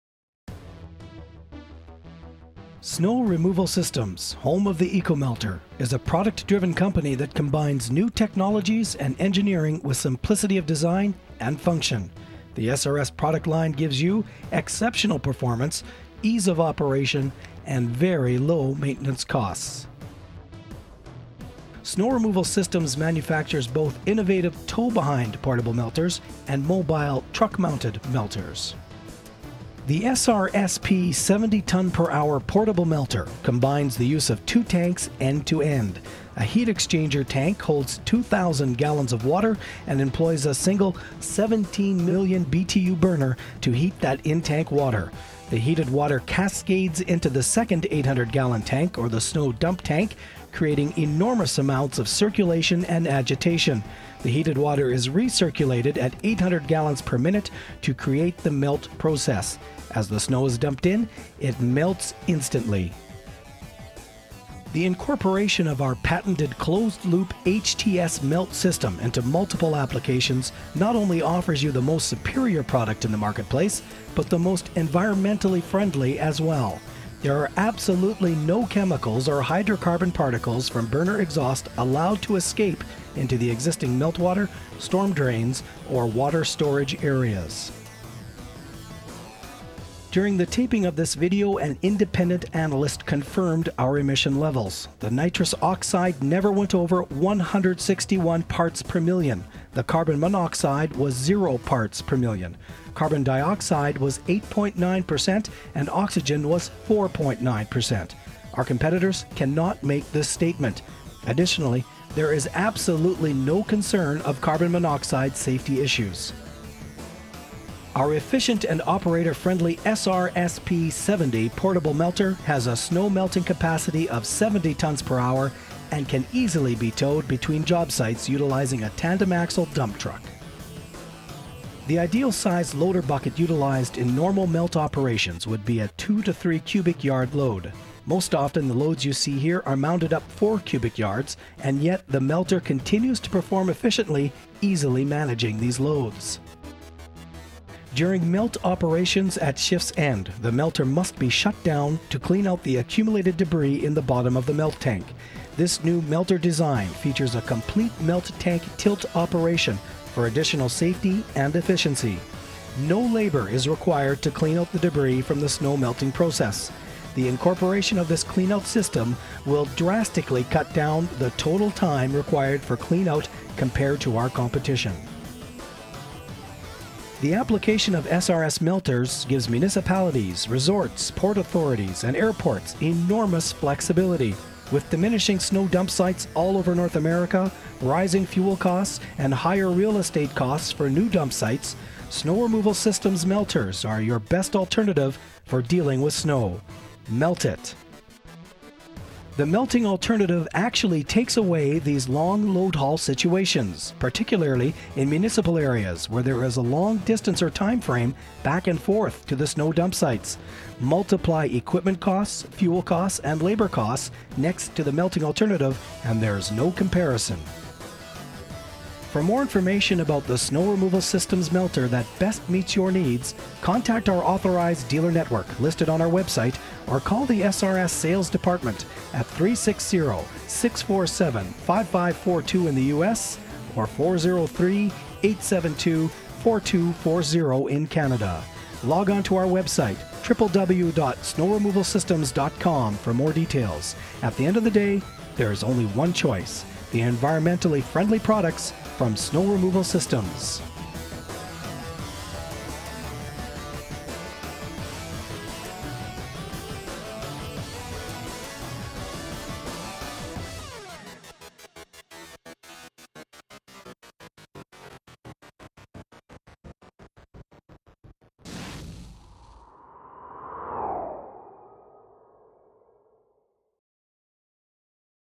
Snow Removal Systems P70-HD.mov
A demonstration of the P-70 snow melter - melt the snow instead of hauling it to snow dump sites.